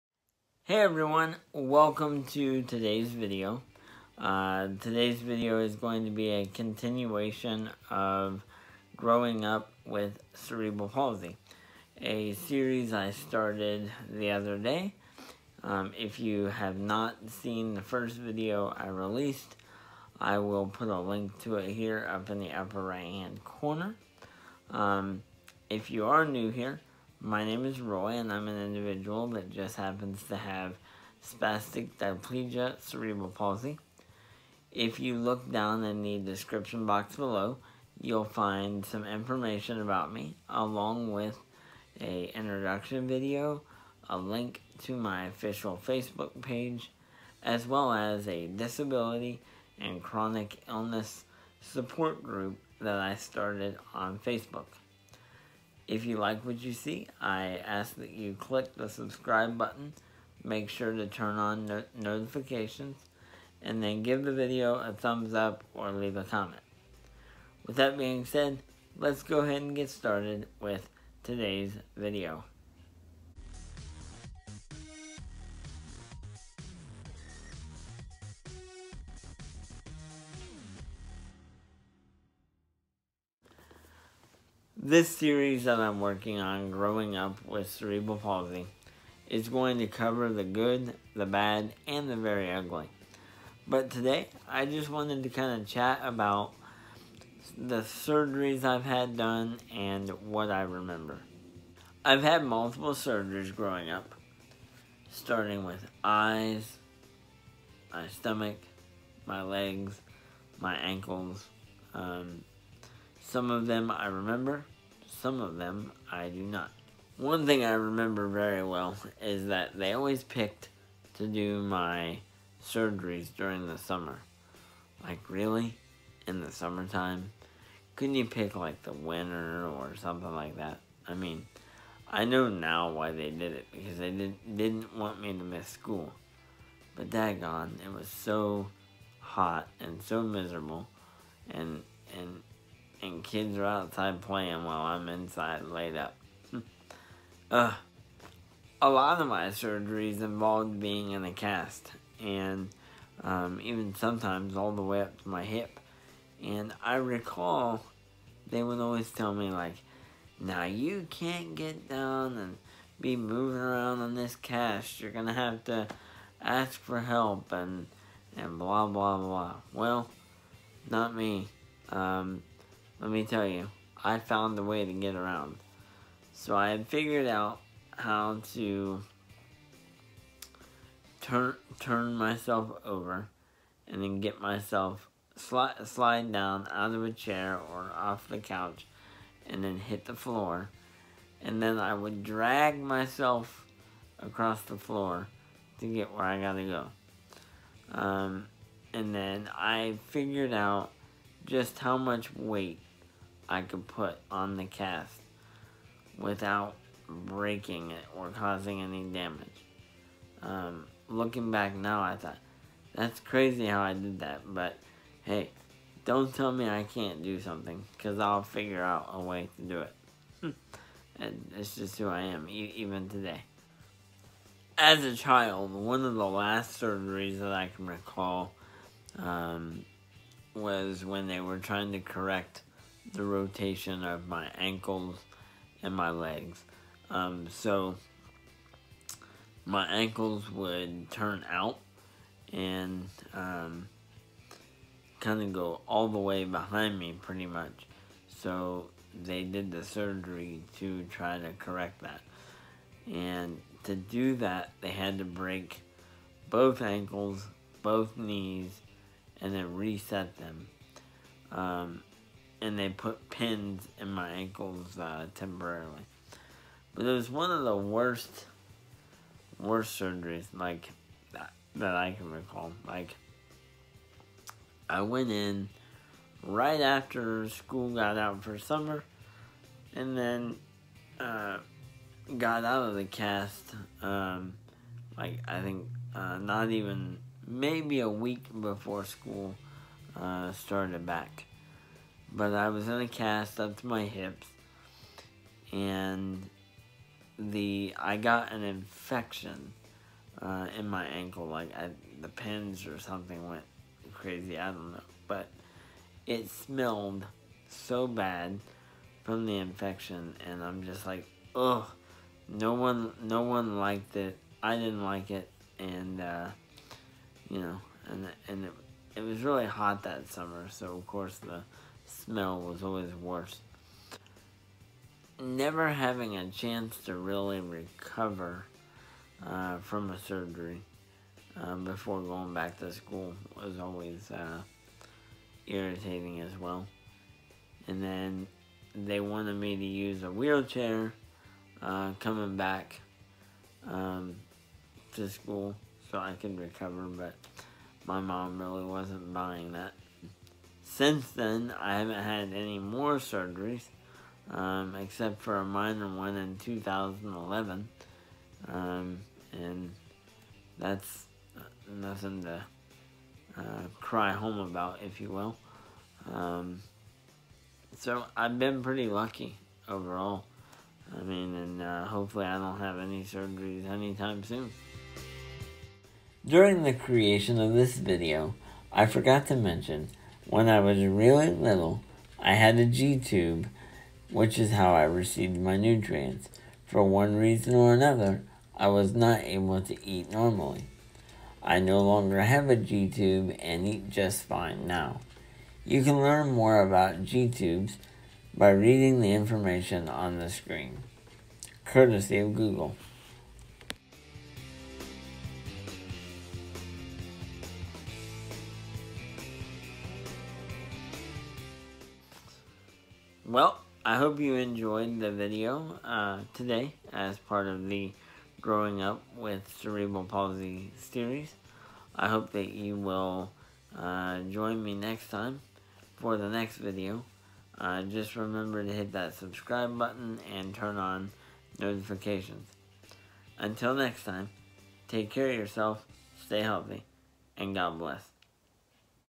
Growing Up with Cerebral Palsy - Surgeries | Roy Morock Jones
In the series of "Growing Up With Cerebral Palsy", I discuss surgeries and my experiences.

My name is Roy, I'm an individual that just happens to have Spastic Diplegia Cerebral Palsy. The purpose of my channel is to help educate and inspire individuals to become their own self-advocate.